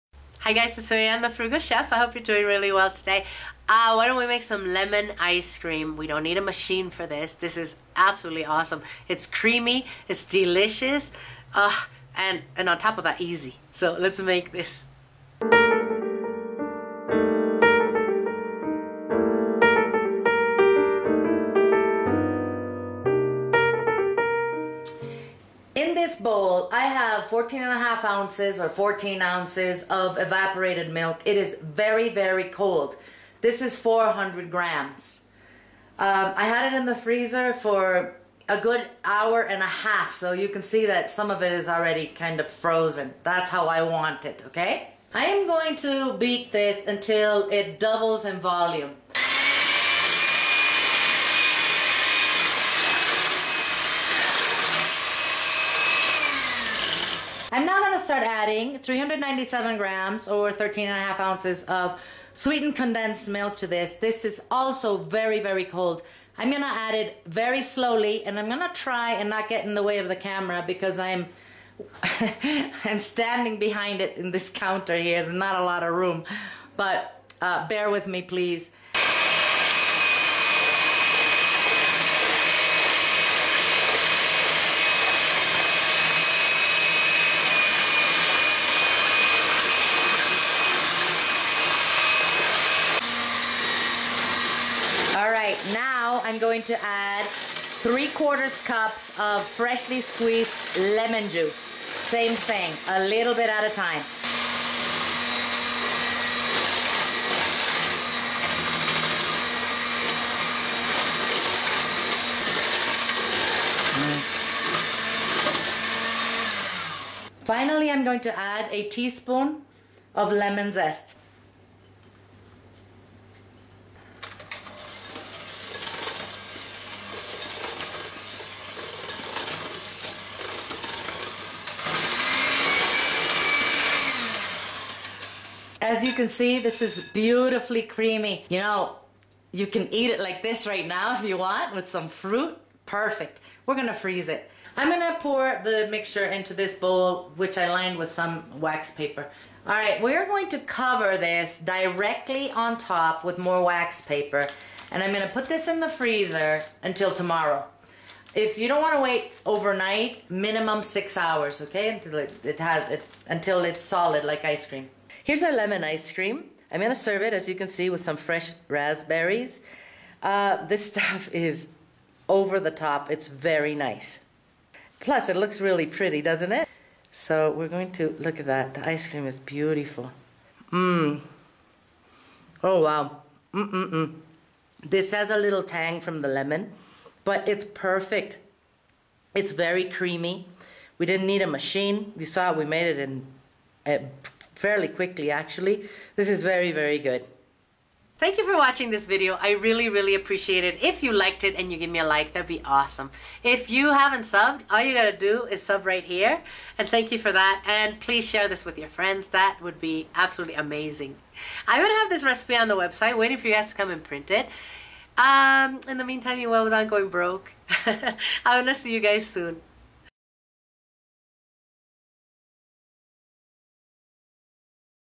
Homemade Ice Cream Recipe (No Machine) -- The Frugal Chef
This homemade ice cream recipe requires no special equipment. We will make it with lemon juice for a slightly tart and very creamy result.

Although the lemons are green please note that they are not limes. However, if you want to make this with limes go right ahead! Great for Memorial Day Weekend!

Serves eight
400 grams (14 oz.) of very cold evaporated milk
397 grams (13.5 oz.) of very cold sweetened condensed milk
3/4 cup freshly squeezed lemon juice
1 tsp. lemon zest

Make sure the evaporated and the sweetened condensed milk are in the freezer for about 90 minutes before you start this. It is okay for the milk to be partially frozen.

Pour the evaporated milk into a large bowl and beat until doubled in volume.

Slowly add the condensed milk and continue beating.  Follow with the lemon juice.

Add the lemon zest and mix in.

Place the lemon cream in a wax paper lined bowl.  Cover the bowl with an extra piece of wax paper placing it directly on to the cream. Freeze for a minimum of 6 hours or up to overnight.

Serve with fresh fruit it you like.

NOTE:  This cream is delicious without freezing as well. We love to serve it with fruit salad or any chopped fruit that is in season.

CALORIES 237.40; FAT 8.84 g (sat 5.43; Mono 2.60; poly 0.27); PROTEIN 7.70 g; FIBER 0 g; CARBS 34.18 g; CHOLESTEROL 44.19 mg; IRON 0.22 mg; Sodium 122.71 mg; CALCIUM 276.12 mg